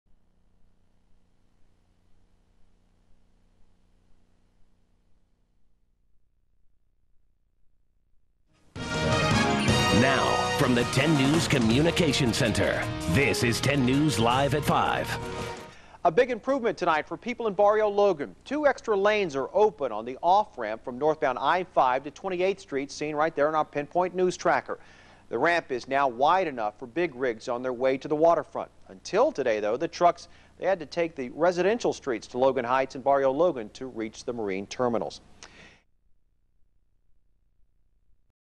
Barrio Logan - 28th Street Traffic Improvements
The Port of San Diego and the Barrio Logan community have been working for years to find solutions to the increasing truck traffic through its neighborhood. As a result of the improvements by CALTRANS recently completed at the I-5 freeway and 28th St., all commercial truck traffic will now access the freeway from 28th St. and 32nd Street instead of driving down Cesar Chavez Parkway. Traffic and land use issues are discussed monthly at the Marine Terminal Community Committee, which is comprised of representatives from the community, the Port of San Diego, the City of San Diego, SANDAG, CALTRANS, BNSF, and local businesses.